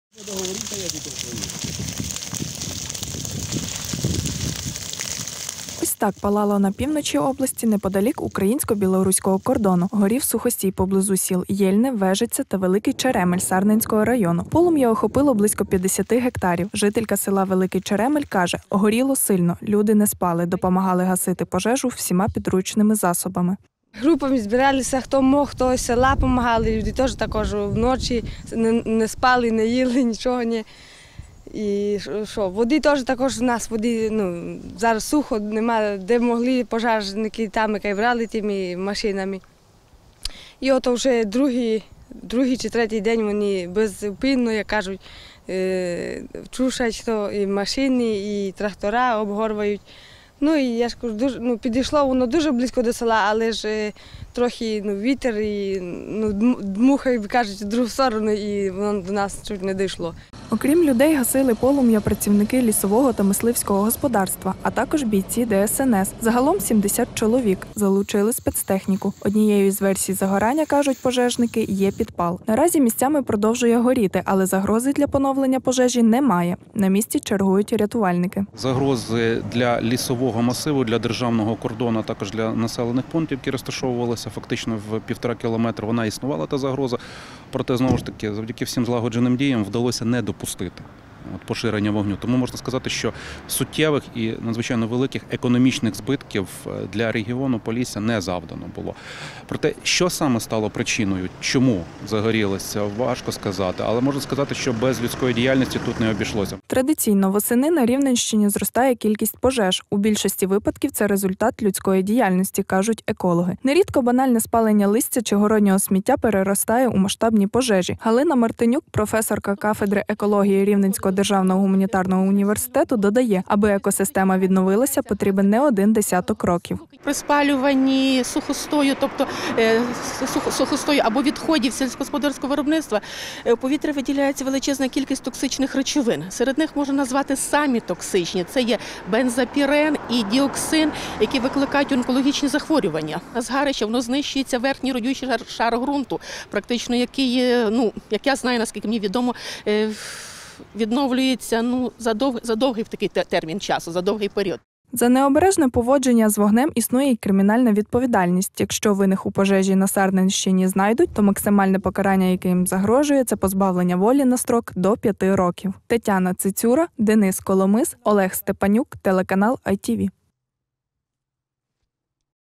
Що стало причиною масштабних пожеж на Рівненщині
Масштабну пожежу сухої трави у полях Сарненського району ліквідували. Причина загоряння ще не встановлена, але рятувальники переконані, що без людських рук тут не обійшлося. Яких збитків завдала пожежа, та як із нею боролися, розкажуть наші журналісти.